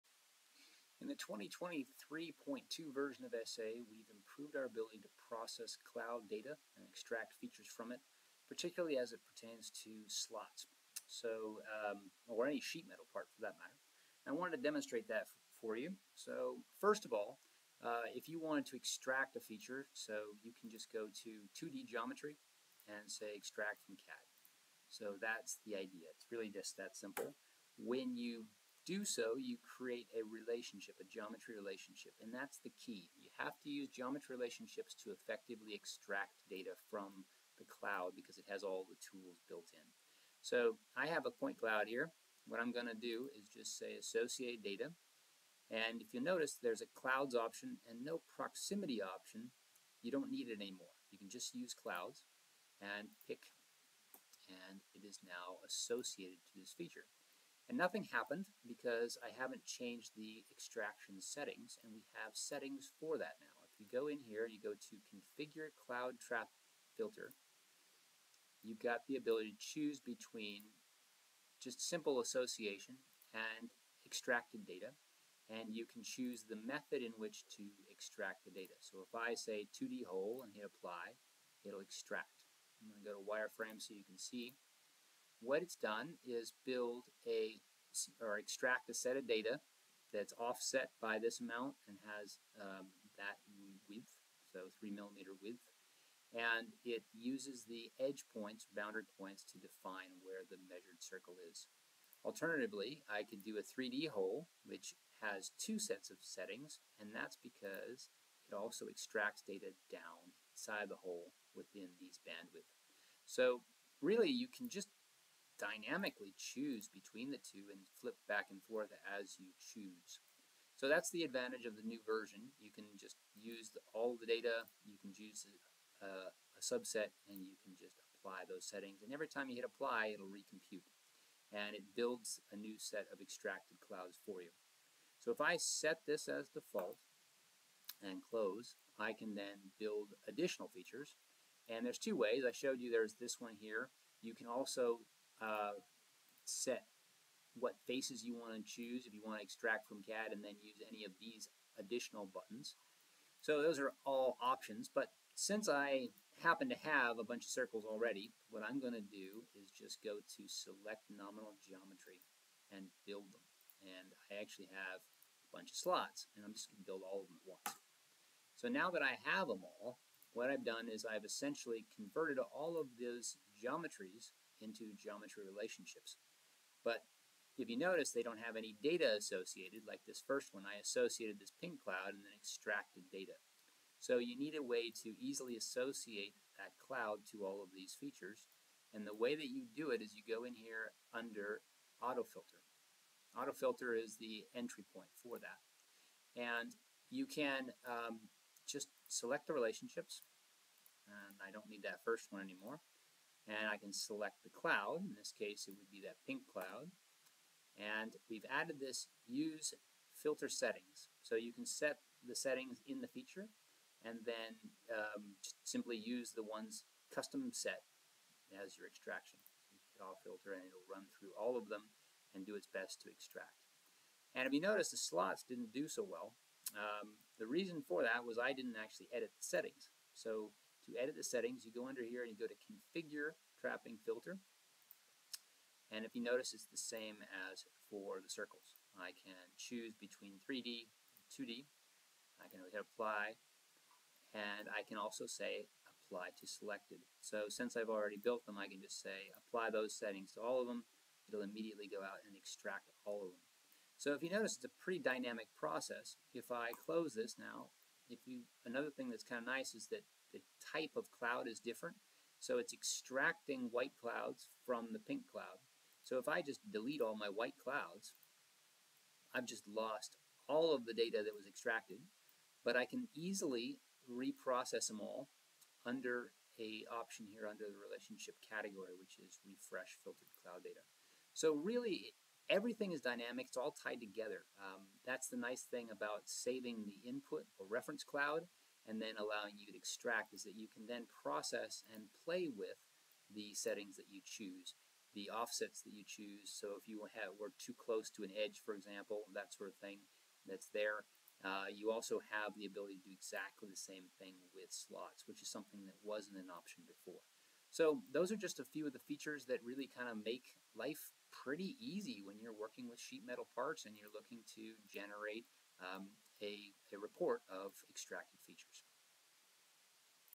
Feature Extraction from Clouds in 2023.2
This video reviews improvements in feature extraction from point clouds available in the 2023.2 version of SpatialAnalyzer